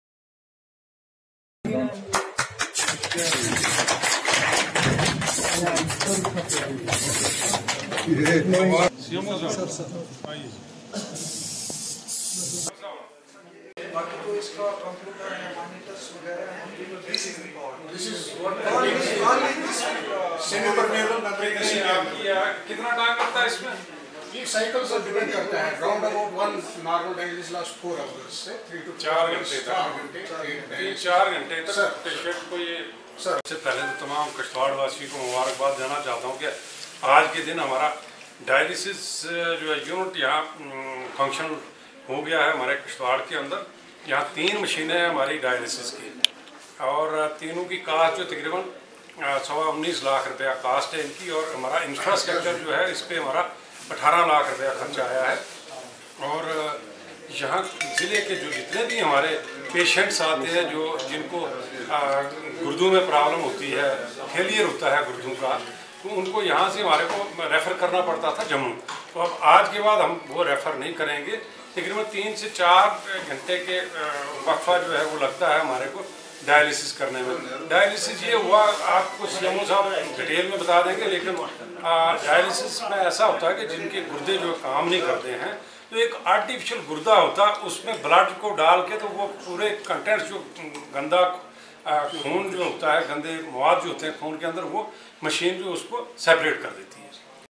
DDC Kishtwar inaugurates Dialysis Centre at District Hospital Kishtwar
chenab News 18